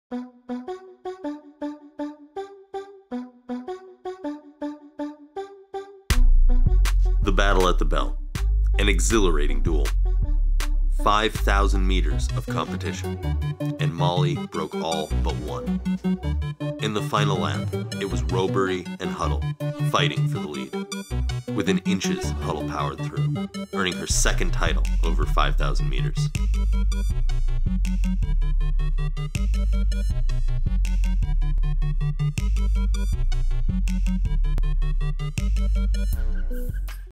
Molly Huddle: RBR American Performance of the Month (June 2014)
RunBlogRun American Performance of the Month
June 2014
Women's 5000m
Molly Huddle vs. Shannon Rowbury
USAOutdoors - Sacramento, CA

At the USA Outdoor Championships two of the top 5000m runners went head to head.  Molly Huddle, the US 5000m record holder had broke the field except for Shannon Rowbury.  It was a battle through the end. Over the final lap Huddle and Rowbury dueled for first, and with 300m to go Shannon kicked past sprinting for the finish, but it was not enough.  Huddle went outside, hitting the finish within a hair in front of Rowbury.